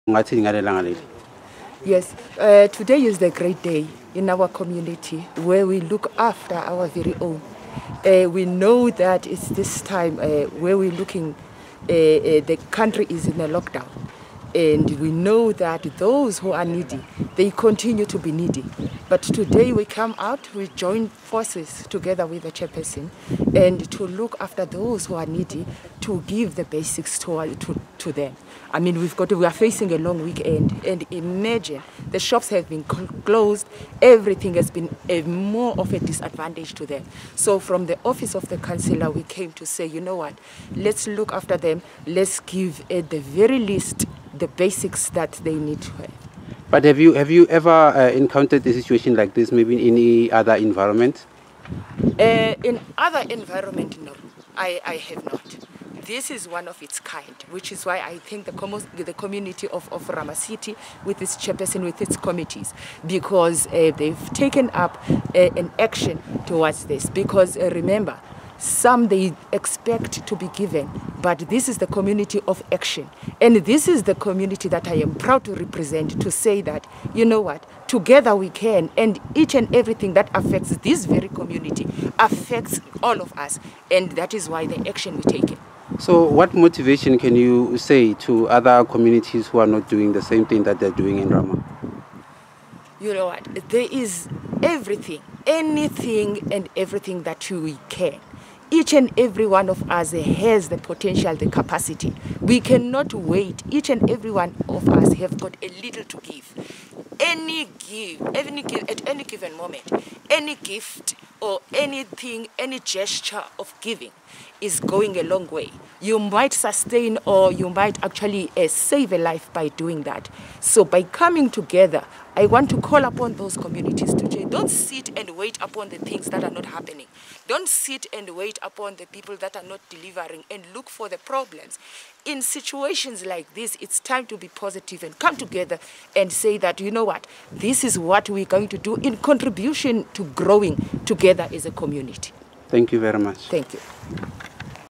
A local foundation hands food parcels over in one of the infromal settlements in ward 4.
Founder of the Overcomer inside out foundation Sipho Mtakati on a food parcel handover in one of the informal settlements in ward 4, north of Pretoria.